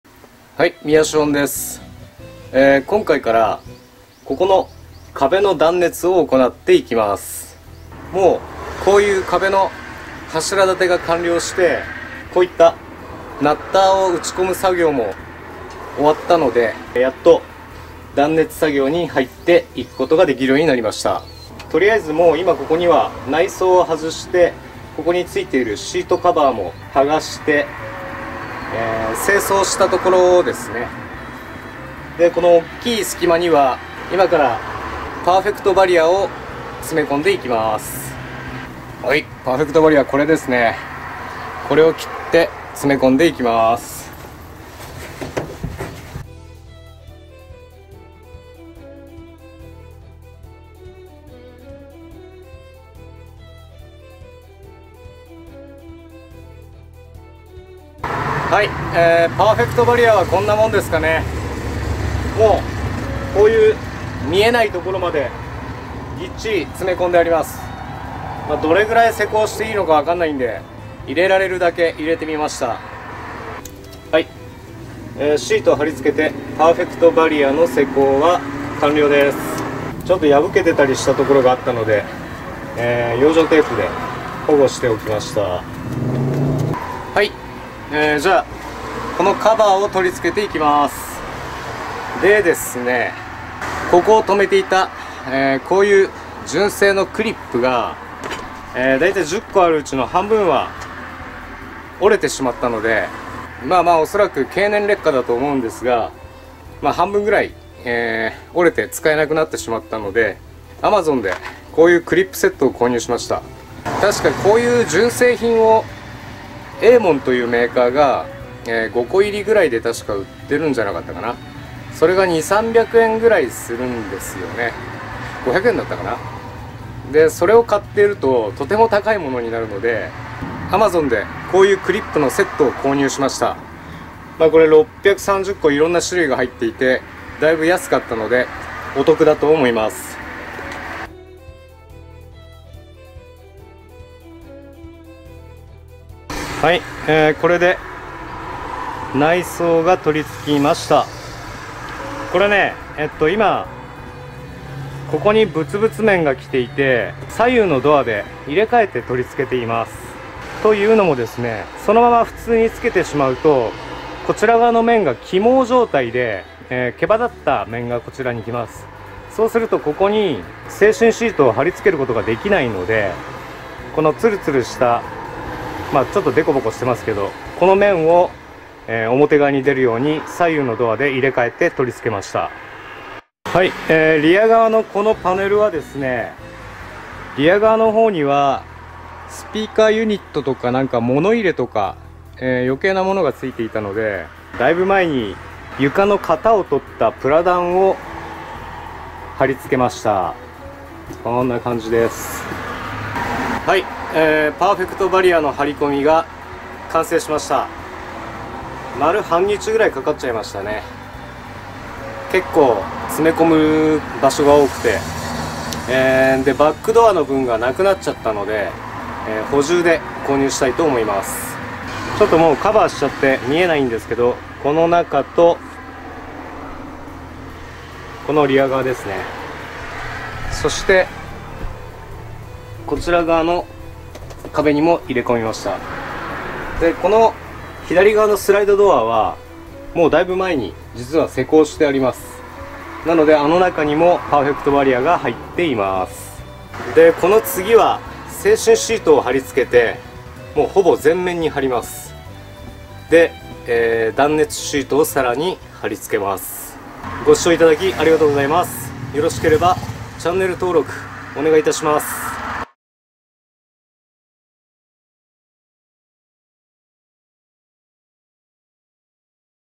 【NV200】Vol.28 壁の断熱！其の1 パーフェクトバリア
やっと壁の断熱に取り掛かりました。
今回はドアのサービスホールや、アウターパネルとインナーパネルの隙間に
パーフェクトバリアを詰め込む作業です。
恐らく皆さんがキャンピングカーDIY動画で見る手法とは違っていると思います。

パーフェクトバリア
クリップ675個16種類

2007年からサイトで発信していましたが、
YouTubeで動画も配信していきます。
動画では語れていない細かな事などもサイトには記載しています。